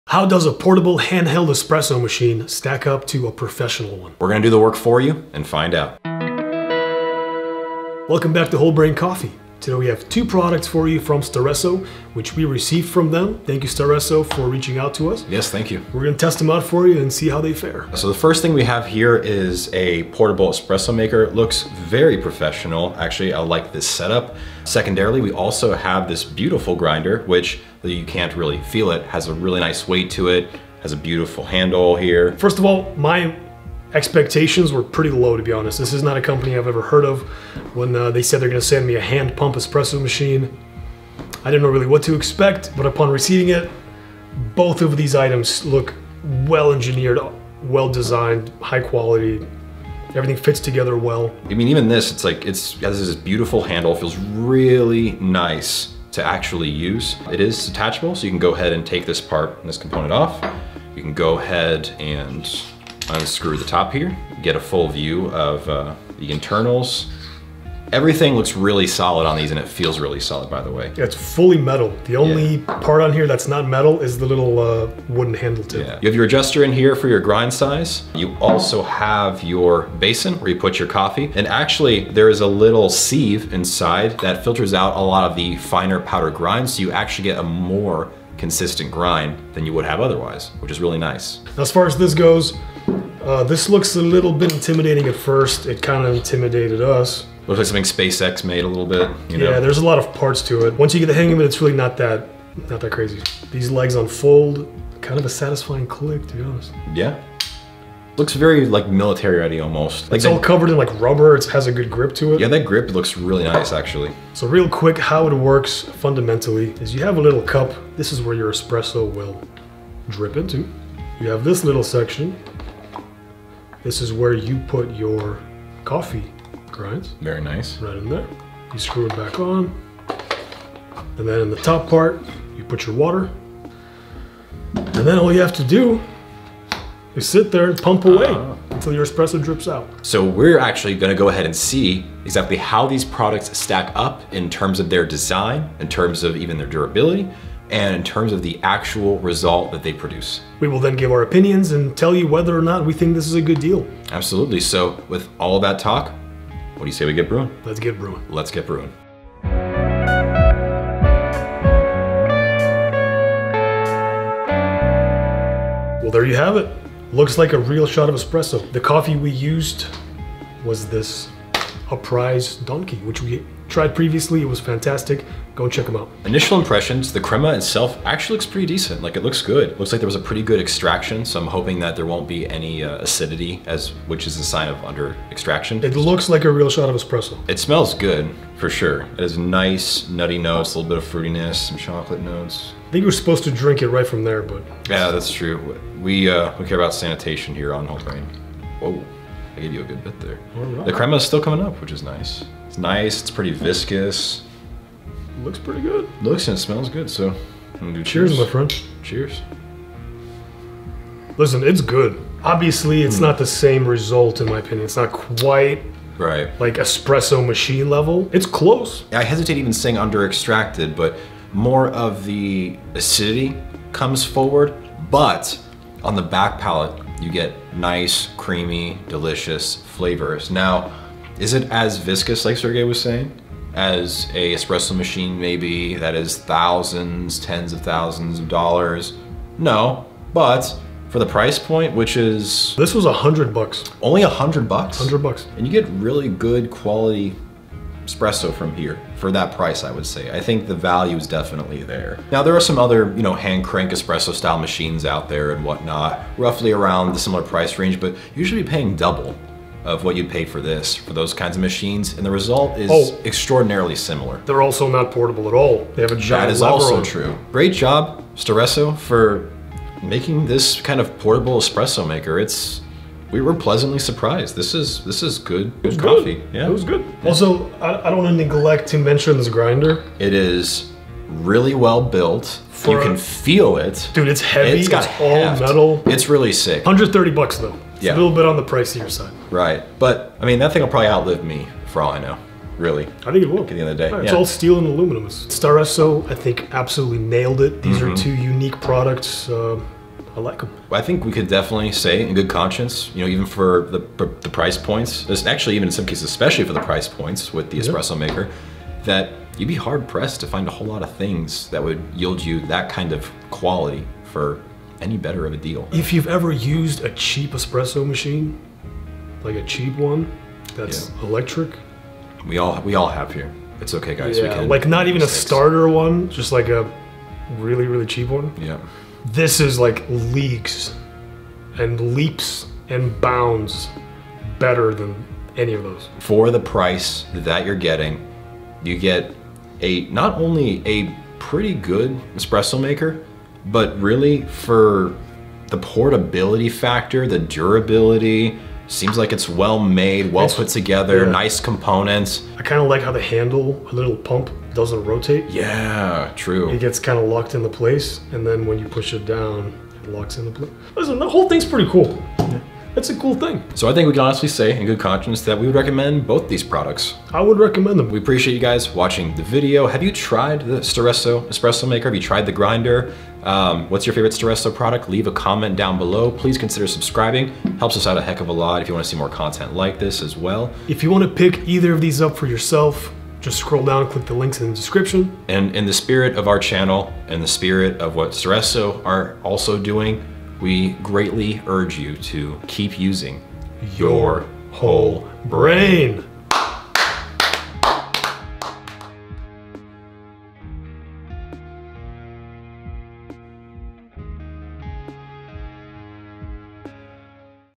This tiny espresso maker OUTPERFORMS the big brands
In this video, we review the Staresso Portable Espresso Machine and the Staresso Discovery Manual Coffee Grinder. Find out how these two products perform and if they're worth the investment for coffee lovers. Watch as we test out the Staresso Portable Espresso Machine and see how it fares against a traditional espresso machine. Find out how easy it is to use the Staresso Discovery Manual Coffee Grinder and get the perfect grind for your preferred brewing method. See the results of our taste test and decide for yourself if these products are worth adding to your coffee arsenal!

☕☕VIDEO CHAPTERS☕☕
0:00 intro
0:51 Initial impressions - Grinder
1:31 Initial impressions - Espresso Maker
2:42 Brewing Results
3:27 Taste Test
5:00 Comparing to other machines
6:01 Do we Recommend?

BTC
bc1qw84x9ahwk4vcnlsuge6dl4z98rvuhps8t973fx
ETH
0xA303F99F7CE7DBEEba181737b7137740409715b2
XMR
86erb5RQPfPVw9M4bN7R31HGT9vWLw51iRce8GykYPfhKiUuzh1Q2hCdsRAaYUQ4RthbD6p8zgfZC7Fiu2tuZXm38WKg266